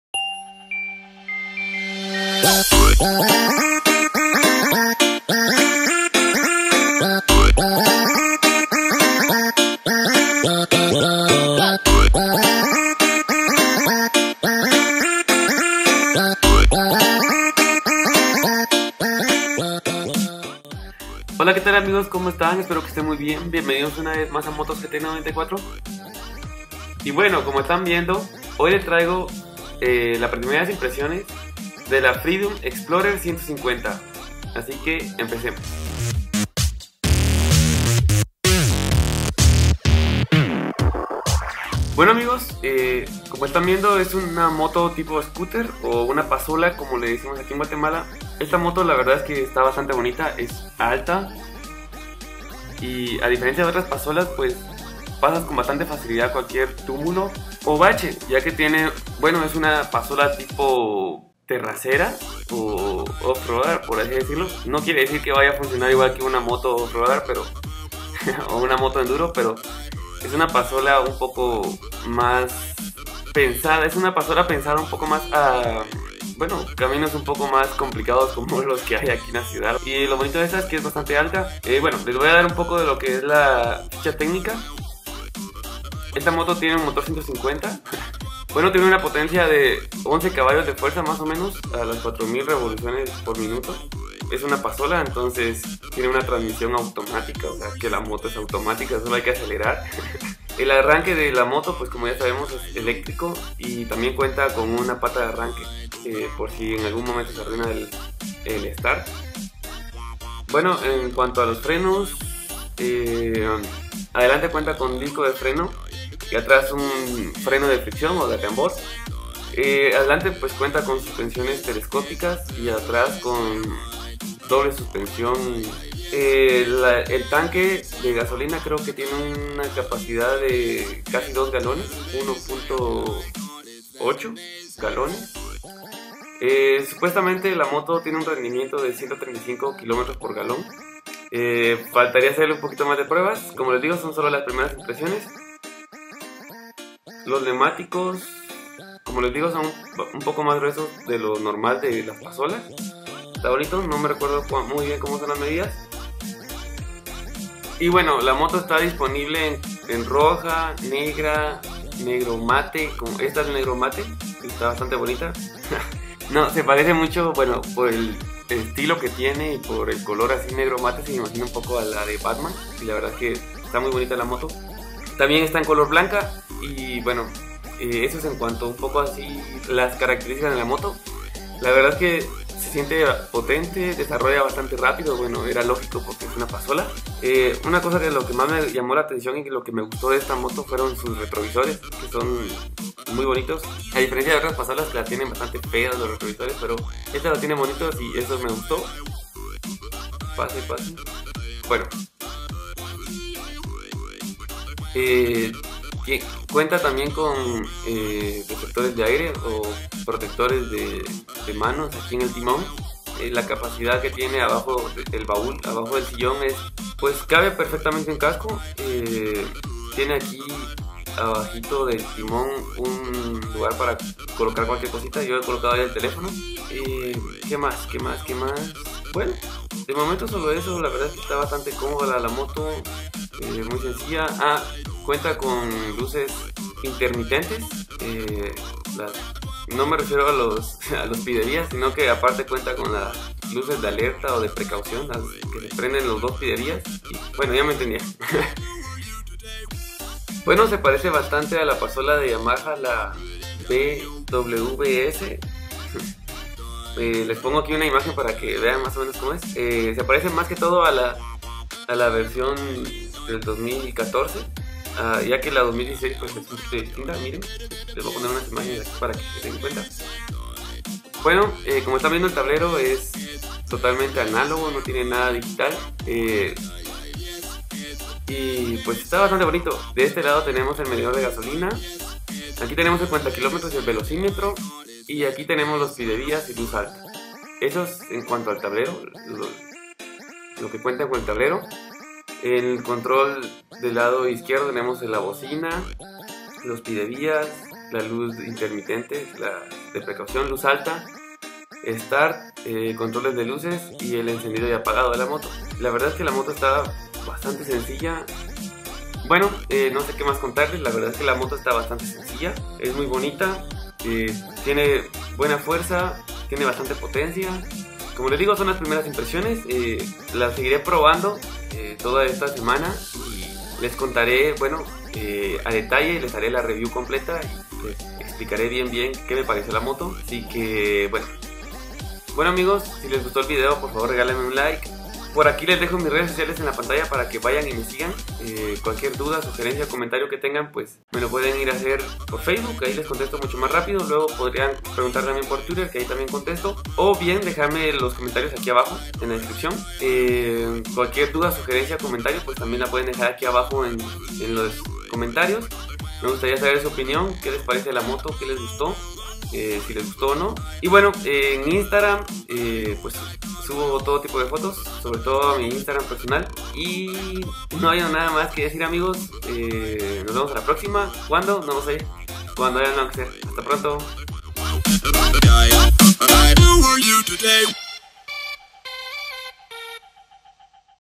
Moto Freedom Explorer 150 - Test Drive - MotoVlog - motosGT94
Solo son las primeras impresiones de la Freedom explorer 150, un scooter muy bonito. Pronto tendrán la REVIEW completa en el canal.

Espero te Guste y si te sirvió de algo el video regalame un LIKE y compartelo con tus amigos.